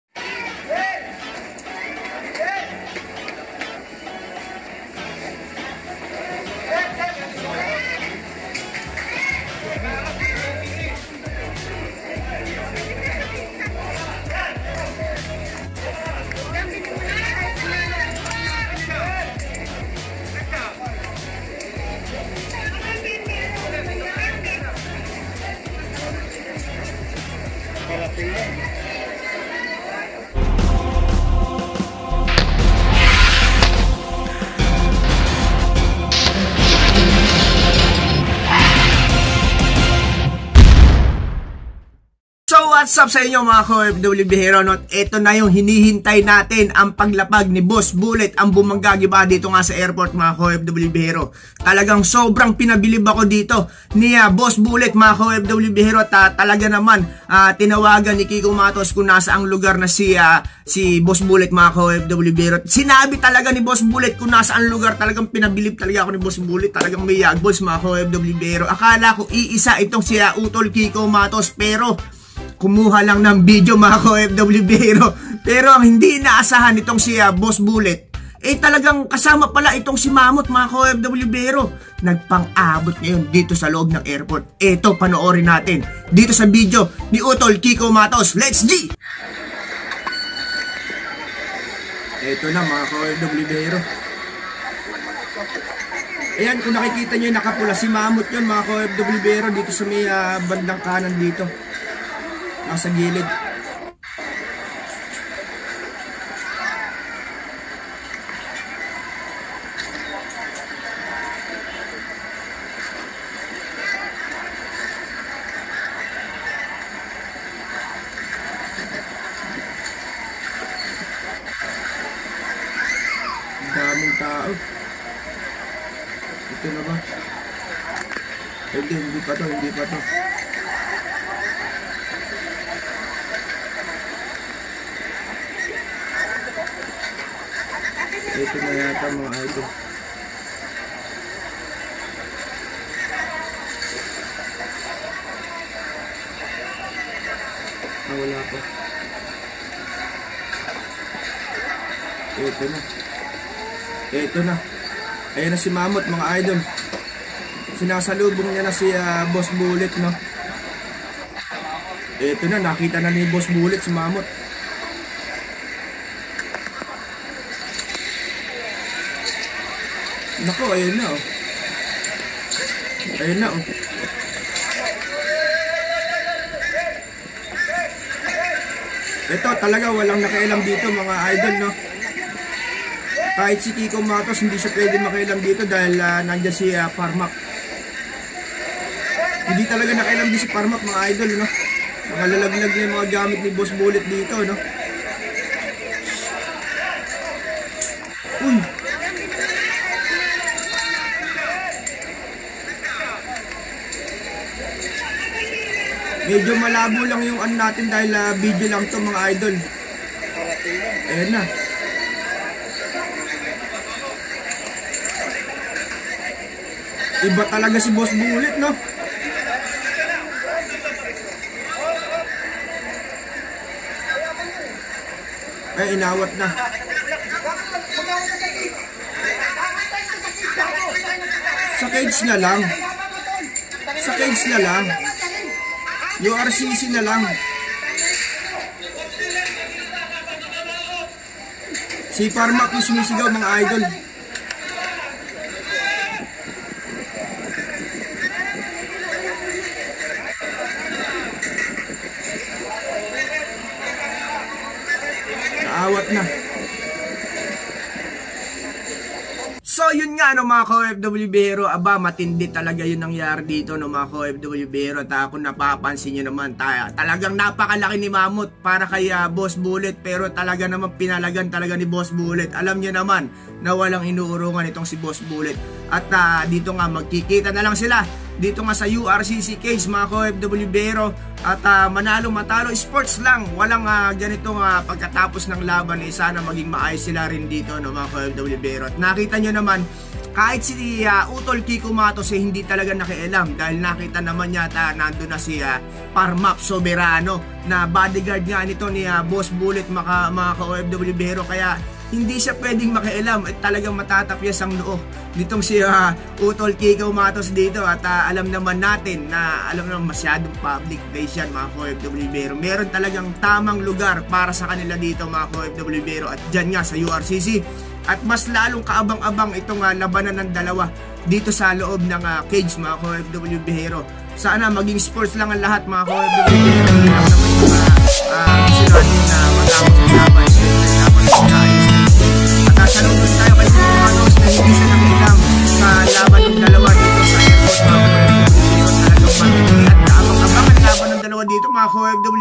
BOSS BULLET AT MAMMOTH NAG TAGPO SA AIRPORT!
Angas mo boss bullet ang tunay na mandirigma!

MAP SoberanoTV